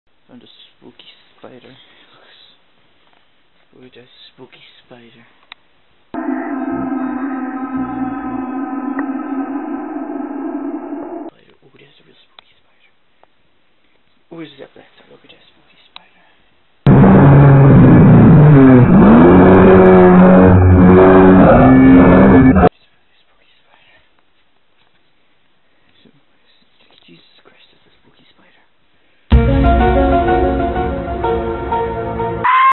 spooky spider
there was a spooky spider in my room this morning and then i couldnt find it so if i die tonight take that spider to court